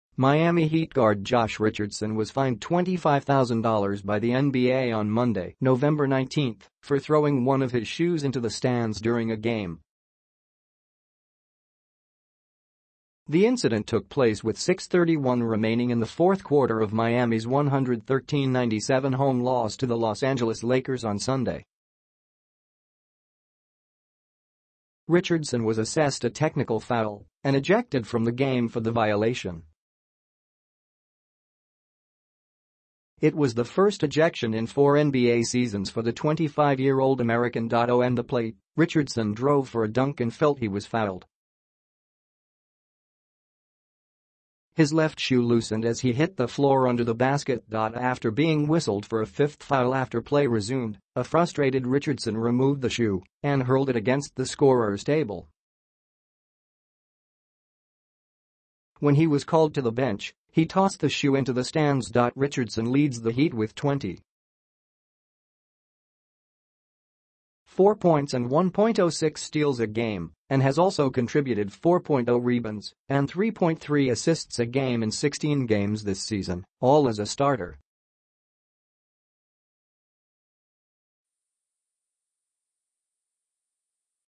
NBA fines Heat's Richardson for tossing shoe into stands
Miami Heat guard Josh Richardson was fined $25,000 by the NBA on Monday, November 19, for throwing one of his shoes into the stands during a game.

The incident took place with 6:31 remaining in the fourth quarter of Miami's 113-97 home loss to the Los Angeles Lakers on Sunday.

Richardson was assessed a technical foul and ejected from the game for the violation. It was the first ejection in four NBA seasons for the 25-year-old American.

On the play, Richardson drove for a dunk and felt he was fouled. His left shoe loosened as he hit the floor under the basket.

After being whistled for a fifth foul after play resumed, a frustrated Richardson removed the shoe and hurled it against the scorer's table. When he was called to the bench, he tossed the shoe into the stands.

Richardson leads the Heat with 20.4 points and 1.06 steals a game and has also contributed 4.0 rebounds and 3.3 assists a game in 16 games this season, all as a starter.

The Heat are 6-10 after two losses in a row.